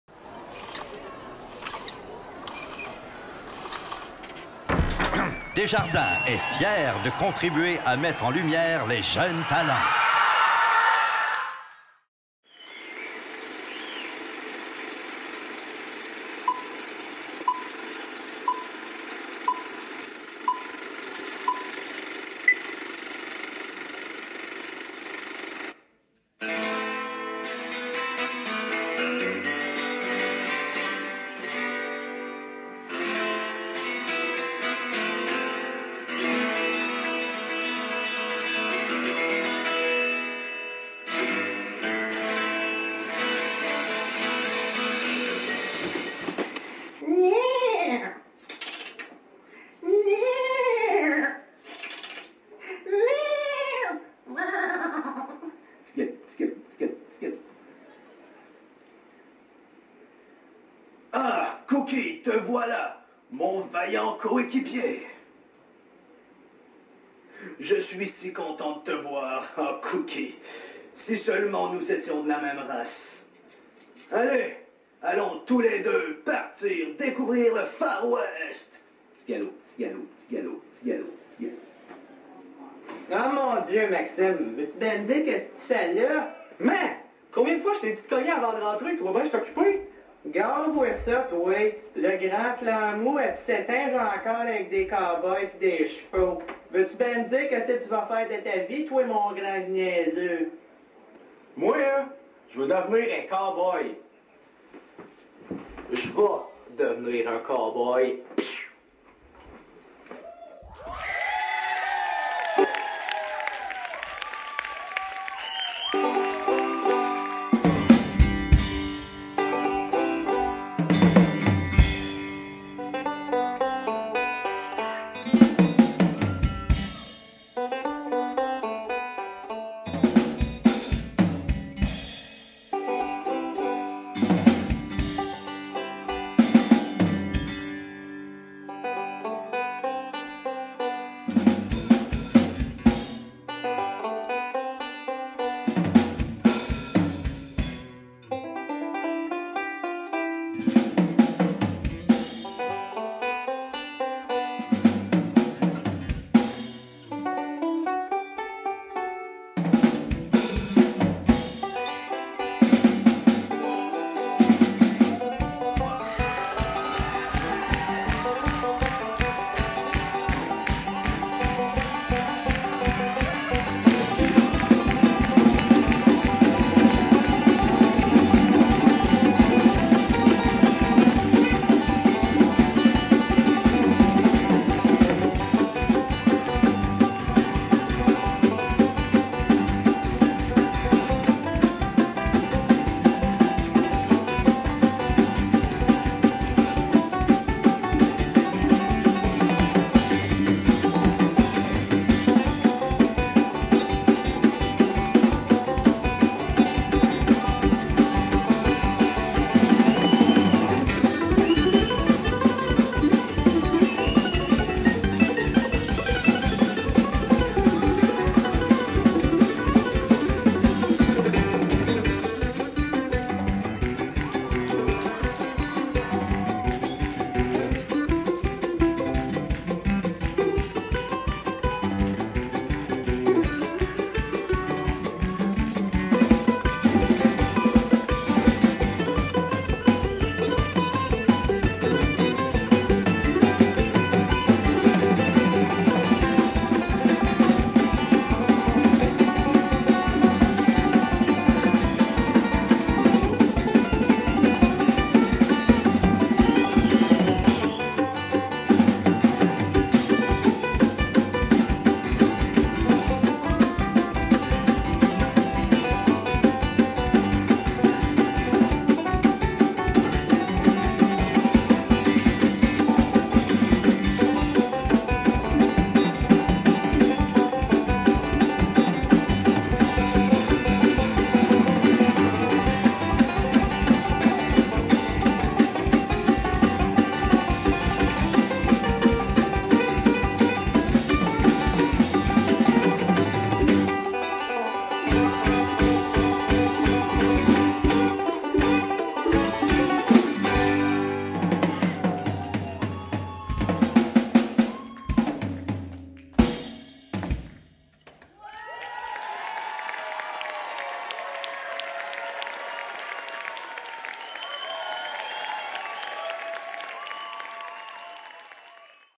Maxime Tessier - Cégep de Sorel-Tracy
Participant: Maxime Tessier
Discipline: Musique et humour (création et interprétation)
Établissement: Cégep de Sorel-Tracy